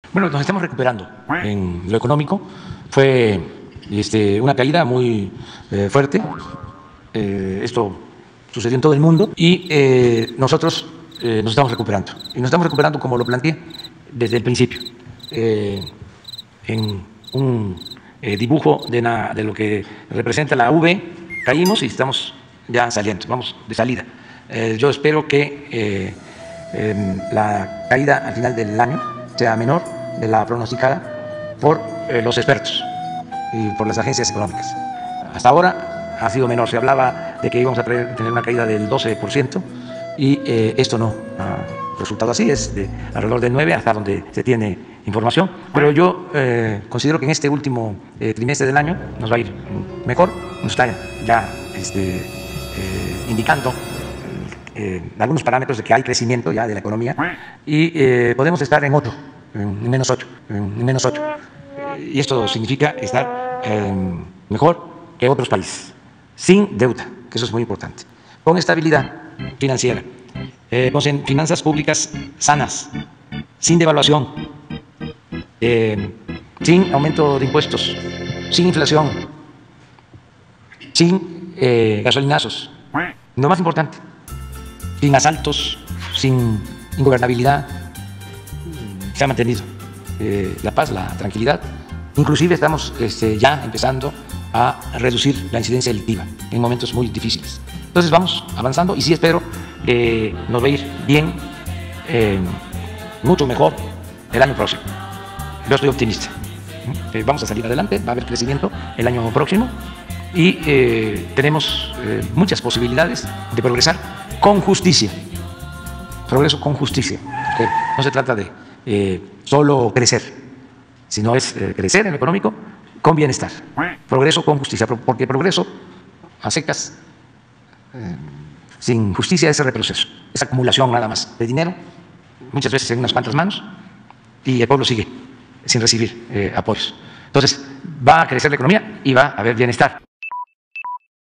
El panorama económico para 2021 según López Obrador 🦆
El panorama económico para 2021 según López Obrador es que vamos a crecer y va haber bienestar 🦆